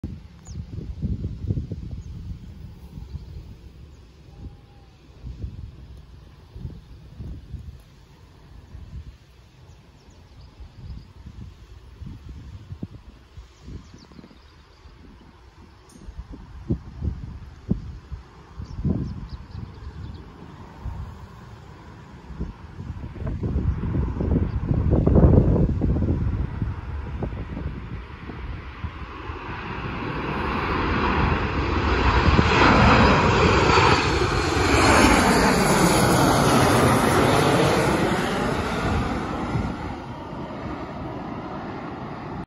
Bonza Airways Landing RW15 At Cairns Airport
Hey all, In todays video was my very first time catching Bonza, after some begging with dad.

Info:
Bonza Airways
AB583
VH-UJT (SHAZZA)
Boeing 737 MAX8
Sunshine Coast - Cairns
Flight Time 2:00